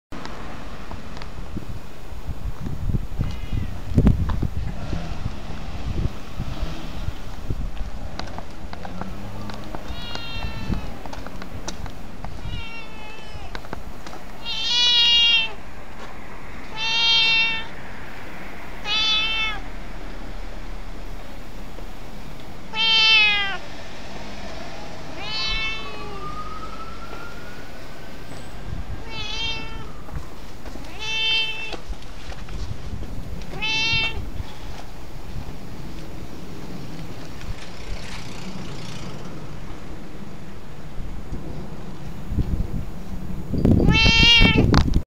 来るのが分かっている猫
久々に出会ったサバトラネコのみーちゃん
私の気配を察知したのかしばらく遠くからガン見していて、近づくと塀の上でダンス（？）します